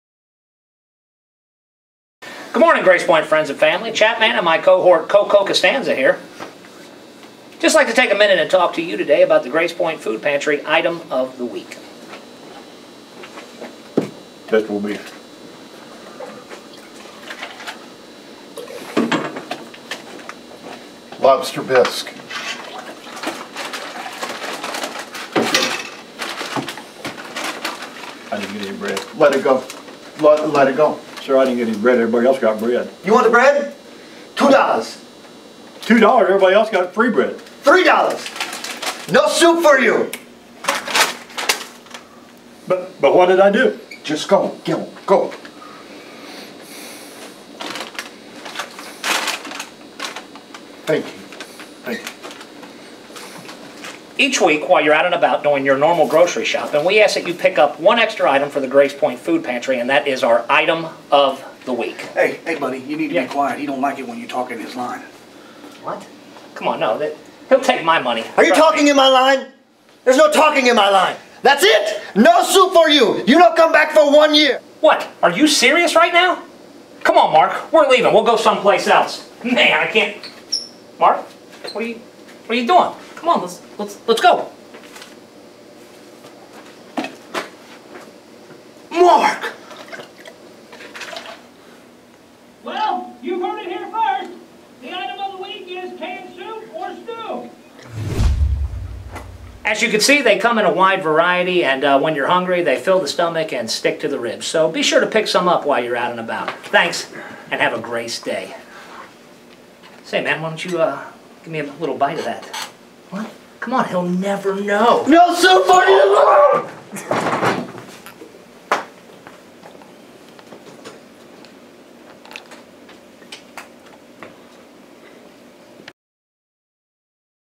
Chapman and Marko Item of the Week Soup
ChapMan and Marko need your help fighting hunger in the Northern Kentucky Area, and so this skit was made for our Food Pantry's Item of the Week to inform and inspire our congregation do that.

Enjoy the laugh!